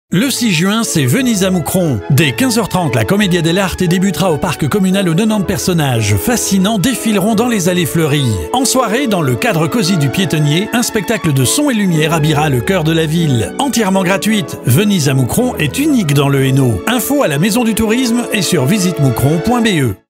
VENISE A MOUSCRON 2015
Le Syndicat d'Initiative de la Ville de Mouscron et la Maison du Tourisme de la Wallonie picarde (Antenne Mouscron) organisent, tous les deux ans, un défilé vénitien lors de "Venise à Mouscron".

 Le samedi 6 juin 2015, Mouscron se mettra à l'heure vénitienne, pour accueillir les célèbres masqués vénitiens.

 Soyez nombreux à venir découvrir les somptueux costumes aux couleurs chatoyantes.

 Cette année, un spectacle haut en couleur et des animations seront au programme.